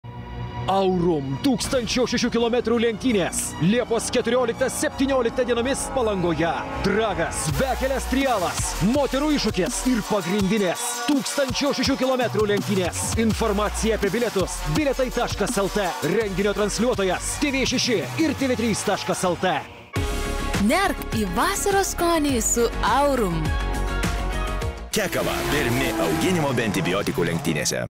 2021 lenktynių anonsas | TV trailer | 2021 Aurum 1006 km powered by Hankook
2021 m. Aurum 1006 km powered by Hankook lenktynės Palangoje vyks Liepos 14-17 dienomis. Kiekvieną vasarą Palanga prisipildo autosporto fanų, kurie nori išvysti geriausius automobilius iš visos Lietuvos ir užsienio. Šiemet šalia didžiųjų 1006 km lenktynių vyks 108 km moterų iššūkis, dragas, Db dragas, žiūrovų slalomas, 4x4 visureigių trialas ir asociacijos „Seni kibirai“ senovinių automobilių paroda!
Aurum 1006 km powered by Hankkok race trailer.